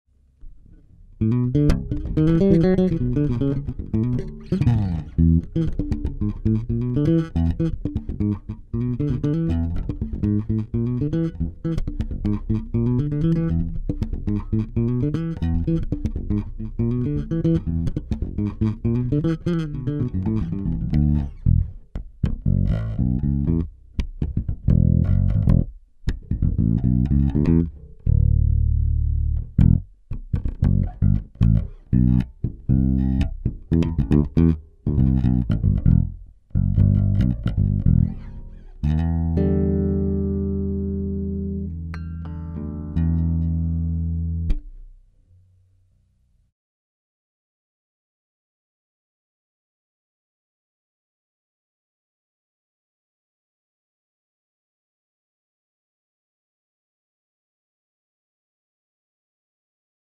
Wood Nico Assumpção Signature - Groove 02
Vídeo curto aonde é demonstrado um pouco do que este instrumento é capaz.

Projeto da Wood Instrumentos Musicais Ltda em parceria com a Carrozza Custom Shop.